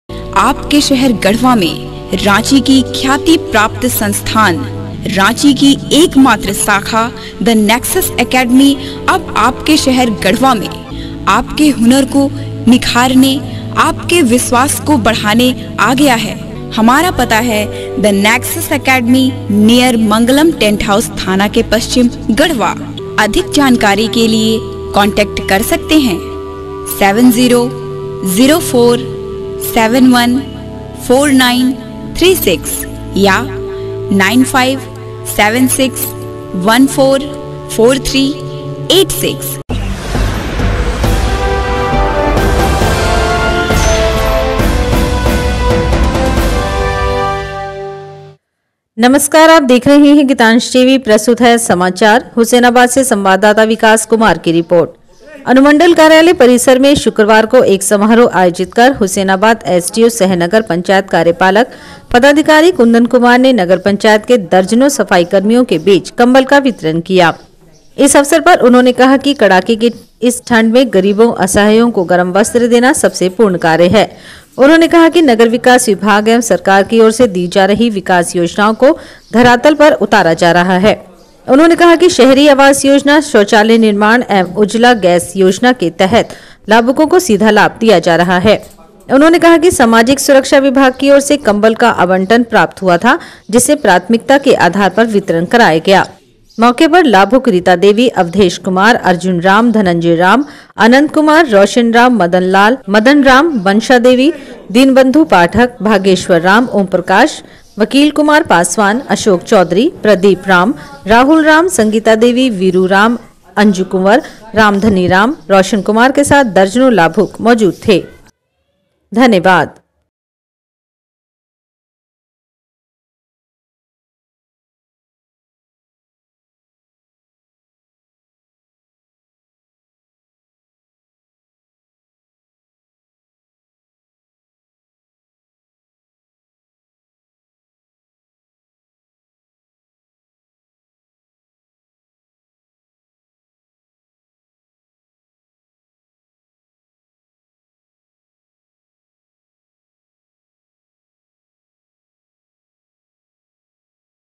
एसडीओ सह कार्यपालक पदाधिकारी ने किया सफाई कर्मियों के बीच कंबल का वितरण
पलामू,हुसैनाबाद - अनुमंडल कार्यालय परिसर में शुक्रवार को एक समारोह आयोजित कर हुसैनाबाद एसडीओ सह नगर पंचायत कार्यपालक पदाधिकारी कुंदन कुमार ने नगर पंचायत के दर्जनों सफाई कर्मियों के बीच  कम्बल का वितरण किया ।इस अवसर पर उन्होंने   कहा कि कड़ाके की इस ठंड में गरीबों, असहायों को गरम वस्त्र देना सबसे पुण्य कार्य है। उन्होंने  कहा कि नगर विकास विभाग एवं सरकार की ओर से दी जा रही विकास योजनाओं को धरातल पर उतारा जा रहा है।